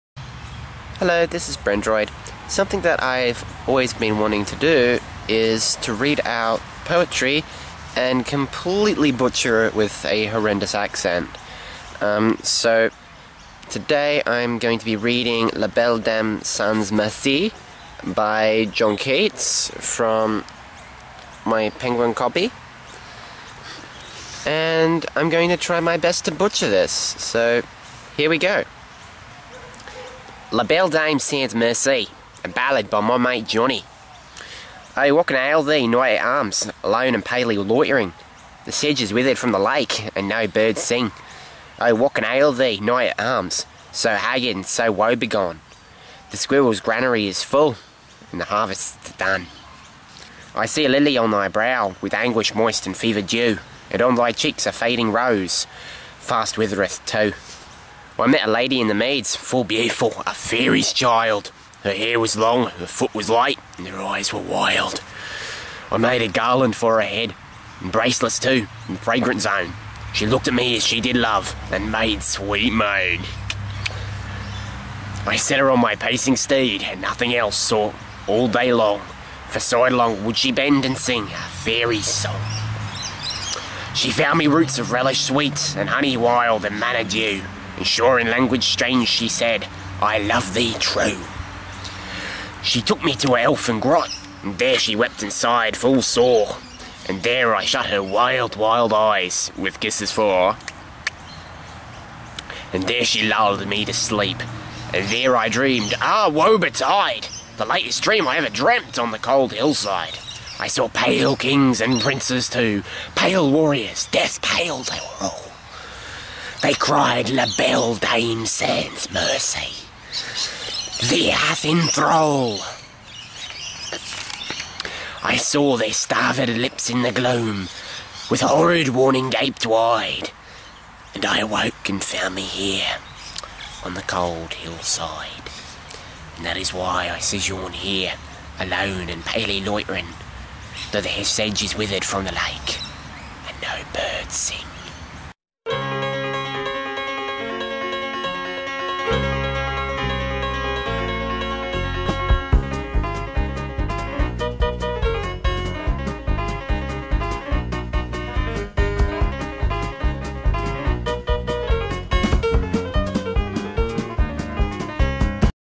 Bogan Poetry Reading: John Keats
So reading classic poetry in a butchered accent has always appealed to me. Here I try reading Keats in a bogan accent!